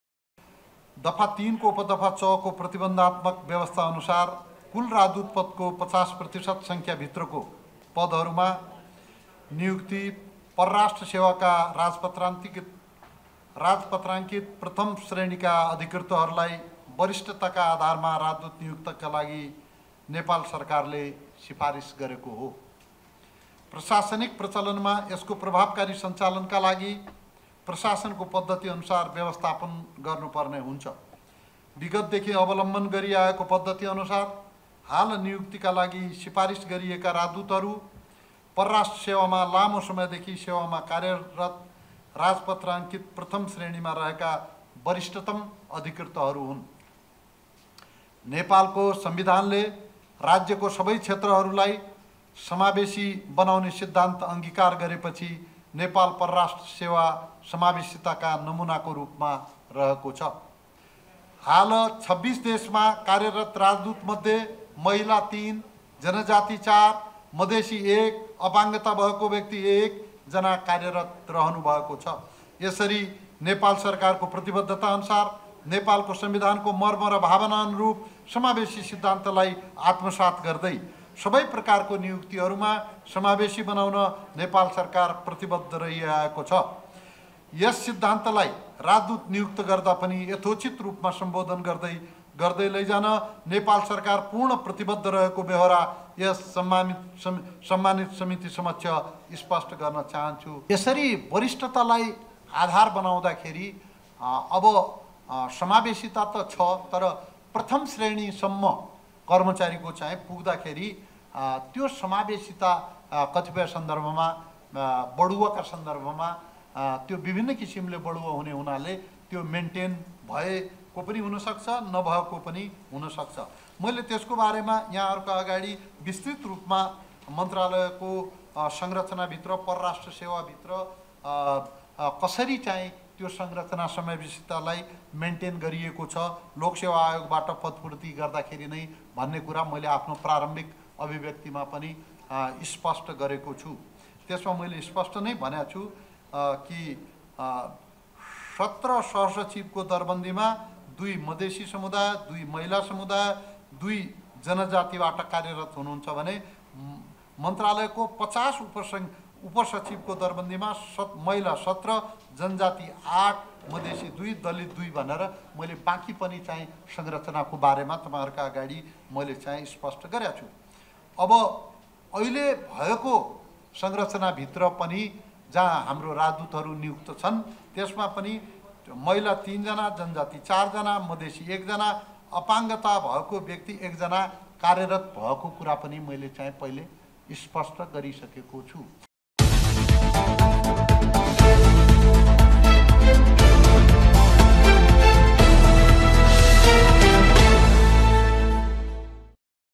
परराष्ट्रमन्त्री एनपी साउदले सबै प्रकारका नियुक्ति समावेशी बनाउन सरकार प्रतिबद्ध रहेको बताए
परराष्ट्रमन्त्री एनपी साउदले सबै प्रकारका नियुक्ति समावेशी बनाउन सरकार प्रतिबद्ध रहेको बताउनुभएको छ ।

बुधबार सिंहदरबारमा बसेको संसदीय सुनुवाइ समितिको बैठकमा राजदूत नियुक्तिका बिषयमा जानकारी दिदै मन्त्री साउदले संविधानको मर्म र भावनाअनुरुप सबै प्रकारका नियुक्तिलाई समावेशी बनाउन सरकार प्रतिबद्ध रहेको बताउनुभएको हो ।उहाले लोक सेवा आयोगबाट पदपूर्तिमा नै समावेशी सिद्धान्तका आधारमा सवैको प्रतिनिधित्व सुनिश्चित हुने गरी दरखास्त आह्वान तथा पदपूर्ति गर्ने गरिएको जानकारी दिनुभयो । राजदूत पदमा ५० प्रतिशत नघट्ने गरी परराष्ट्र सेवाका कर्मचारीलाई नियुक्त गर्नुपर्ने निर्देशिकाको प्रावधान पालना गर्दै अहिले राजदूत सिफारिस भएको उहाले बताउनुभयो । विशिष्टिकृत योग्यताका आधारमा परराष्ट्र सेवाका कर्मचारीलाई राजदूत सिफारिस गरिकै कारण अहिले पूर्ण रुपम समावेशी हुन नसकेको उहाले बताउनुभयो । मन्त्री साउदले हाल २६ देशमा कार्यरत राजदूतमध्ये महिला तीन, जनजाति चार, मधेसी एक र अपाङ्गता भएका एक जना रहेको पनि जानकारी दिनुभयो ।बैठकमा समिति सदस्यहरुले संविधानको व्यवस्था अनुसार समावेशीका आधारमा राजदूत सिफारिस गर्न सरकारको ध्यानाकर्षण गराएका थिए ।